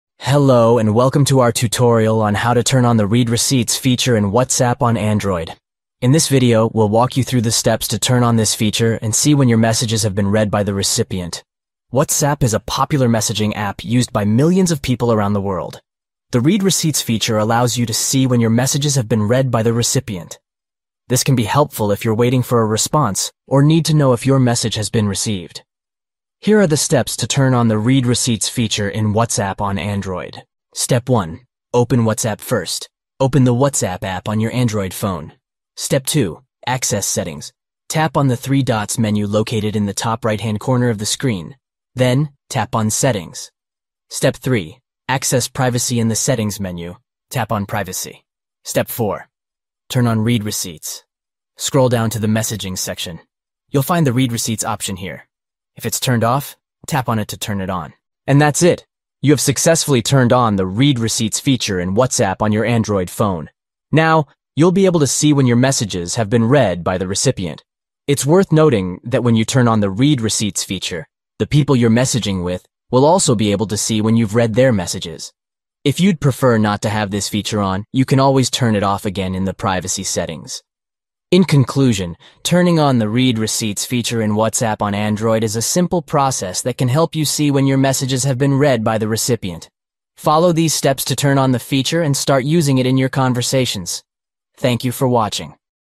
How To Turn On The Read Receipts Feature In WhatsApp
Learn how to turn on the Read Receipts feature in WhatsApp on Android with this easy tutorial. See when your messages have been read by the recipient by following these simple steps.

Step 1: Open WhatsApp
Step 2: Tap on the three dots menu located in the top right-hand corner of the screen
Step 3: Tap on "Settings"
Step 4: Tap on "Privacy"
Step 5: Scroll down to the "Messaging" section
Step 6: Toggle on the switch next to "Read Receipts"

By following these steps, you can turn on the Read Receipts feature in WhatsApp on your Android phone. Now, you'll be able to see when your messages have been read by the recipient.

Watch this tutorial and learn how to turn on the Read Receipts feature in WhatsApp on Android. See when your messages have been read by the recipient and stay on top of your conversations.